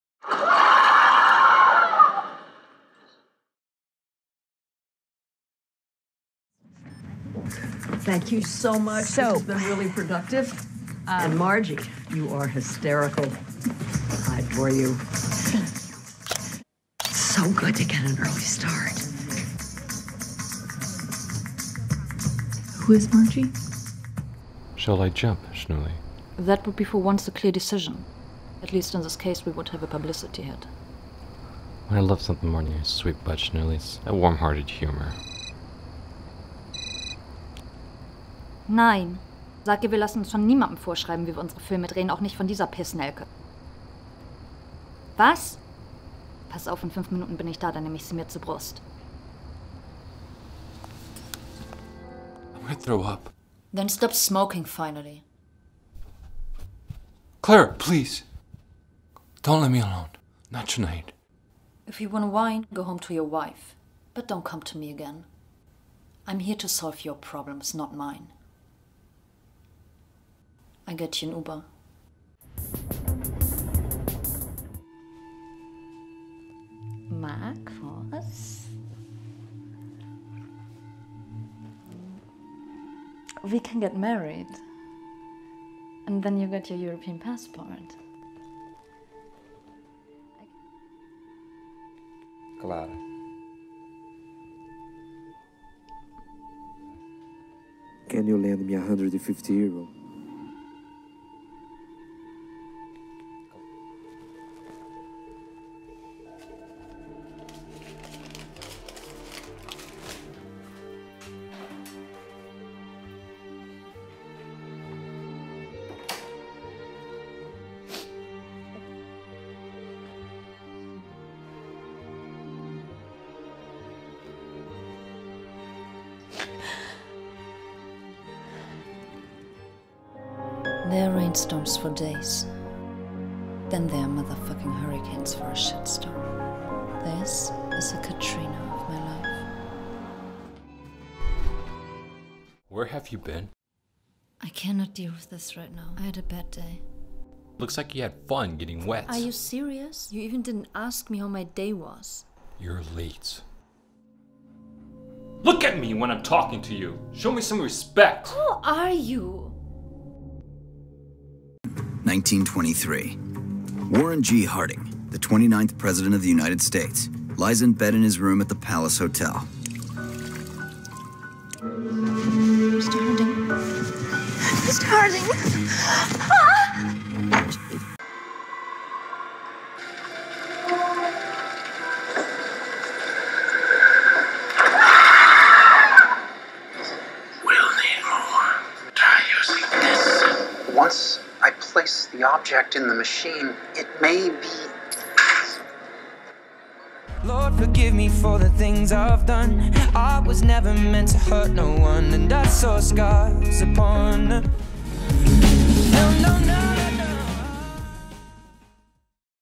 Romika Annabell • Theatrical Acting Showreel
Romika Annabelll is an American actress, born in Germany. Annabell's edgy European look, the piercing blue eyes, her enormous emotional strength and variability makes each of her performances unique.